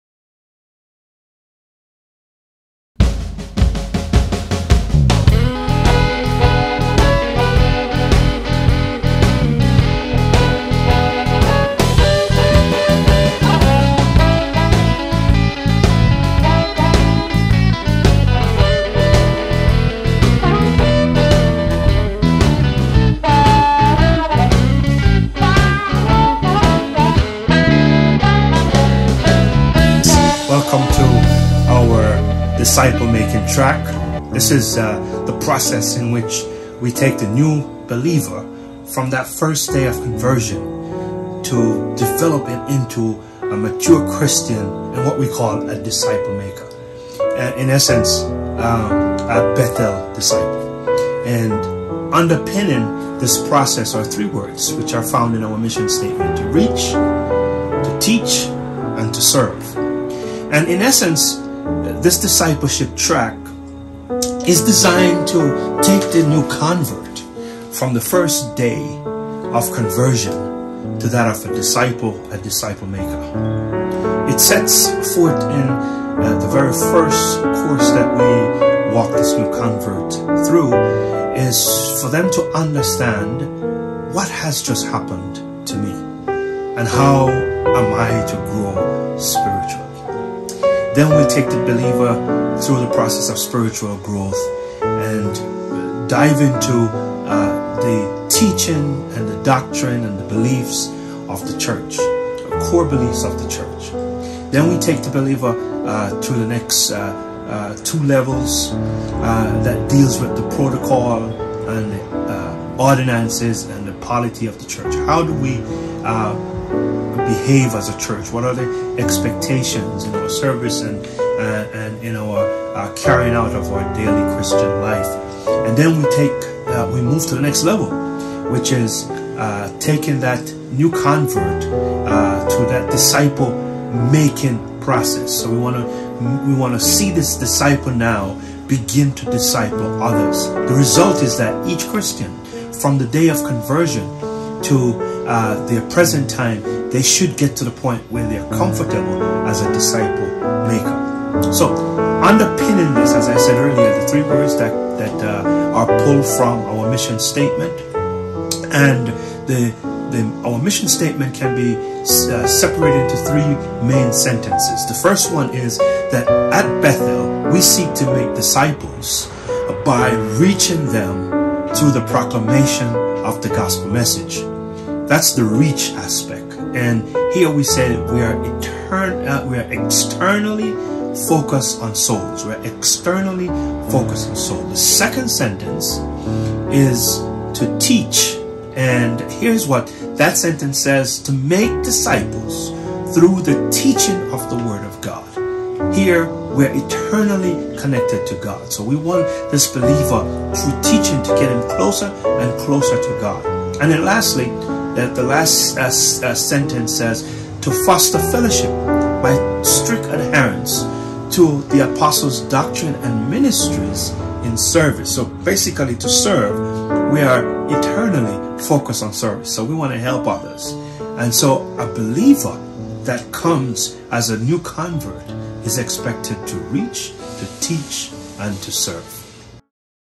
The Making of a Bethel Disciple
This discipleship track is design to take the new convert from that first day of conversion to a disciple.  It sets fort in course1 to help the new believer to understand what has just happened to them and how to grow. The spiritual growth of the convert is outlined in course 2.  It identifies the core beliefs of the church. The believer is led to another level in chapters 3 & 4 that deals with the protocol and ordinances of the church.  Then the section on Discipleship takes the believer to the next level of a disciple-maker.  The result is that each Christian should grow into a mature disciple.